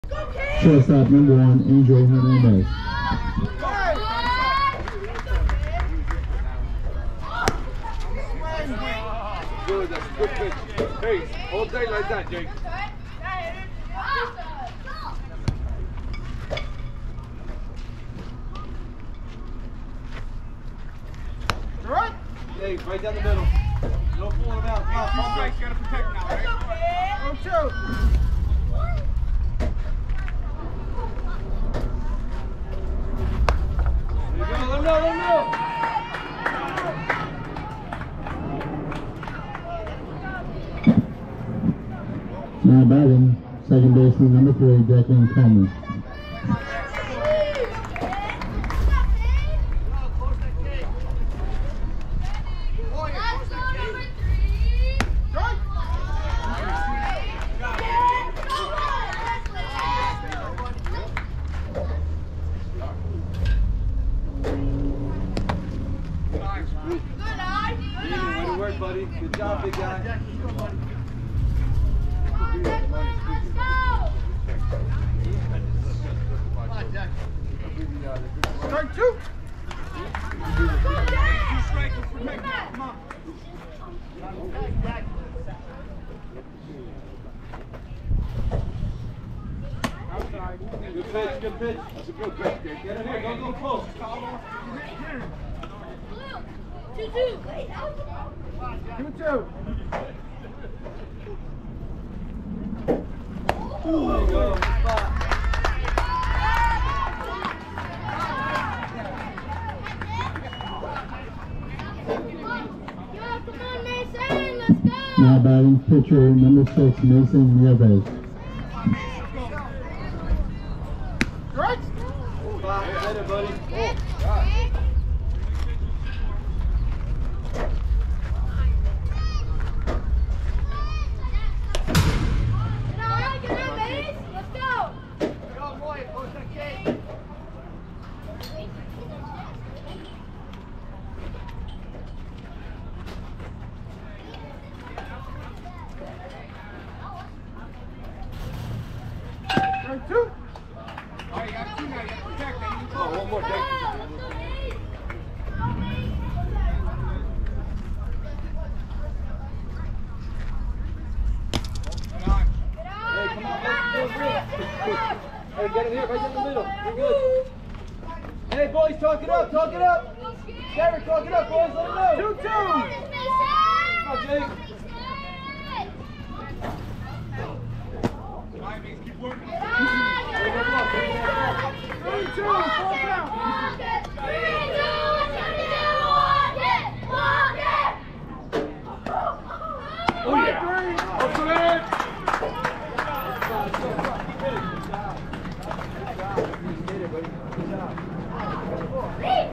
Jake pitching vs Kearny 10U Allstars
3rd inning of the game Jake pitched awesome. Went 1,2,3 in 14 pitches throwing heat.